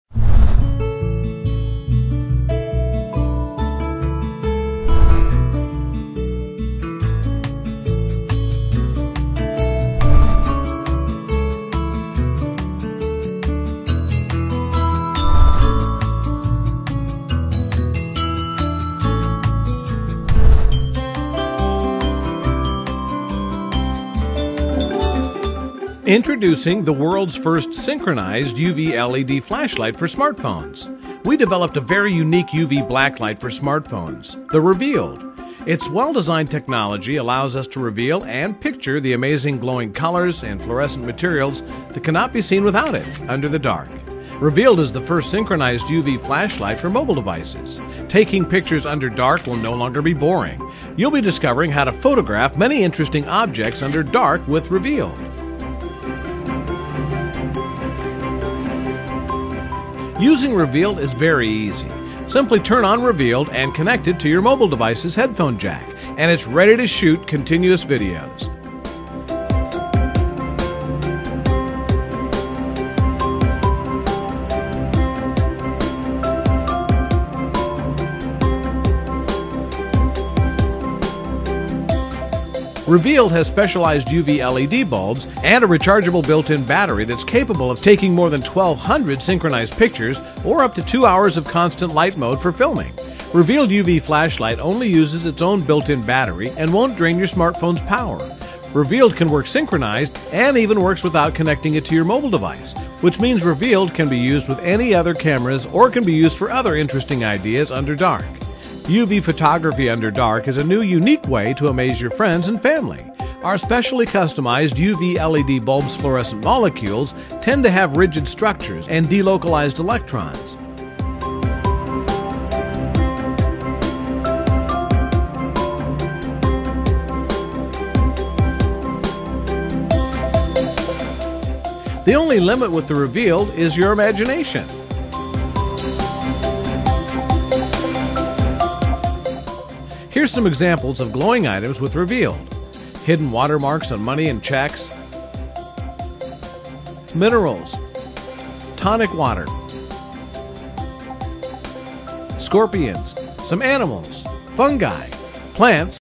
UV LED Flash Light for Smartphones - ReveaLED
ReveaLED is the world's first synchronized UV LED flash light for smartphones and tablets. ReveaLED fits to the audio jack of your device for discovering UV light photography under dark. It has a built-in rechargeable lithium-ion battery that means you can also use ReveaLED freely without connecting it to your device.

Enjoy ;0)